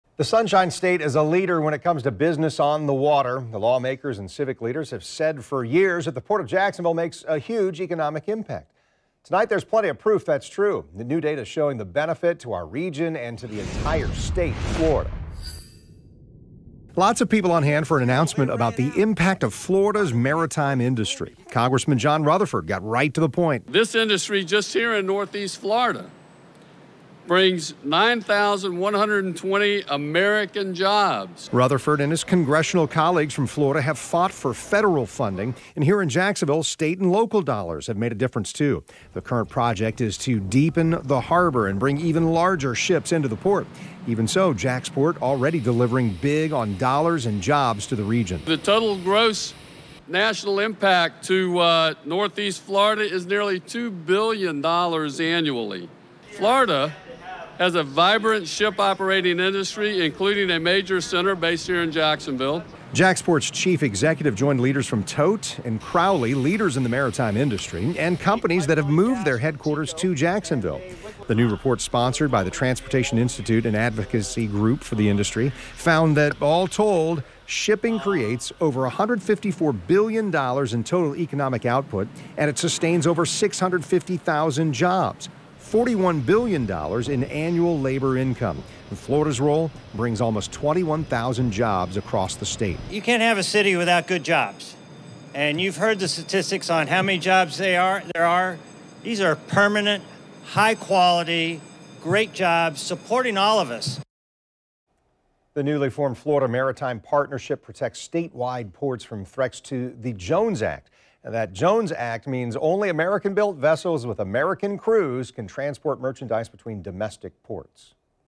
JaxPort delivers big on dollars and jobs to Northeast Florida region
The Sunshine State is a leader when it comes to business on the water.